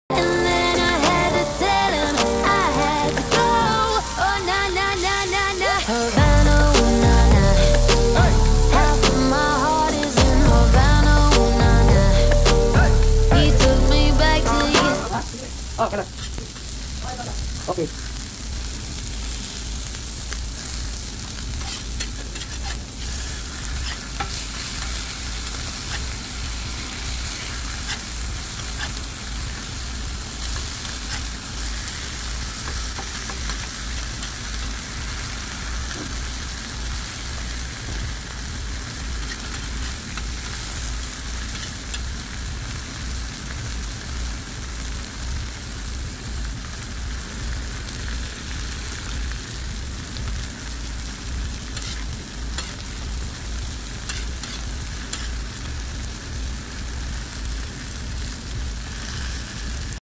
BEST FISH FOR GRILL SEABREAM RICH IN OMEGA3#shorts
sea bream is good for the heart rich in omega3, our best seller good for grill